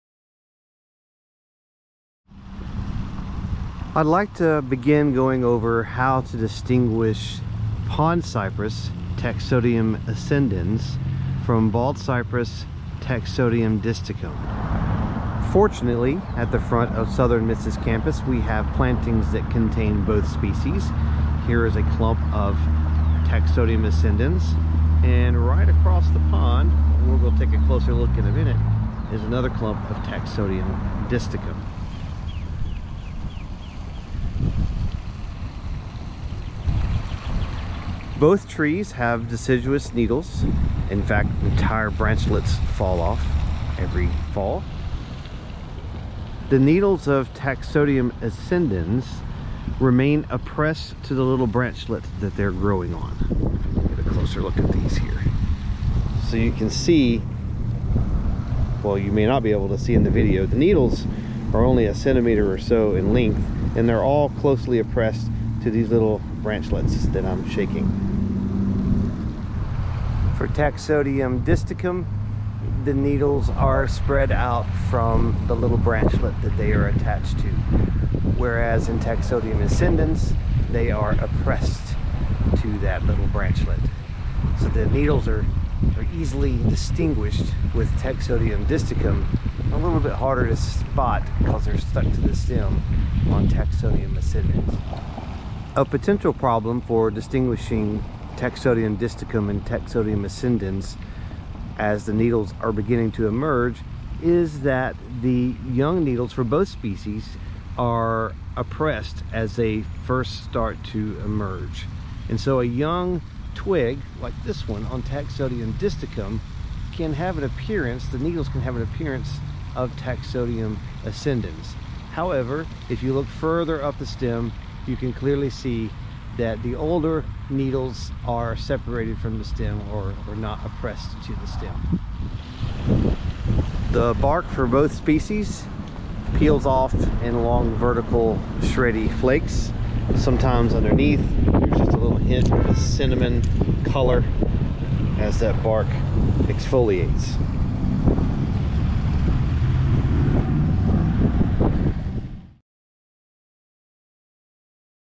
Taxodium distichum vs Taxodium ascendens
Taxodium distichum and Taxodium ascendens are very similar, however, the needles for each are quite distinctive and can be used to easily distinguish them.